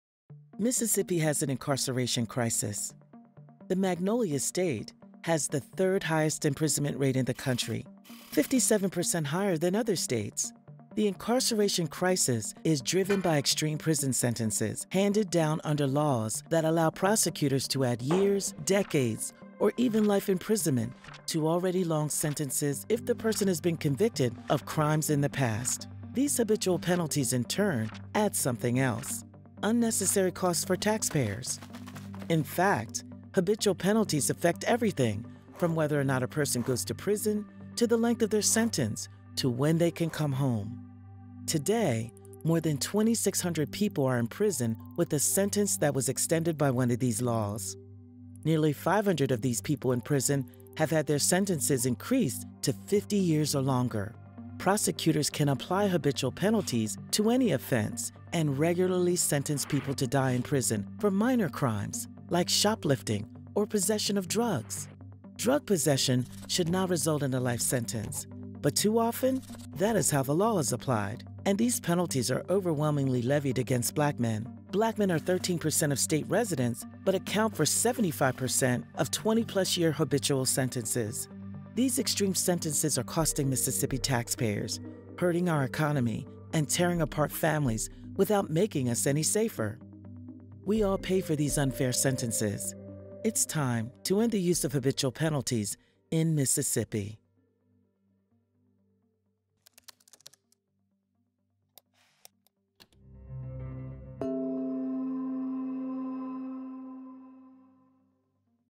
WeAllPay — Mississippi's harmful habitual laws
Habitual laws add extra years, decades, or even life imprisonment to prison sentences if a person has been convicted of crimes in the past. These laws directly contribute to Mississippi having the third-highest imprisonment rate in the country.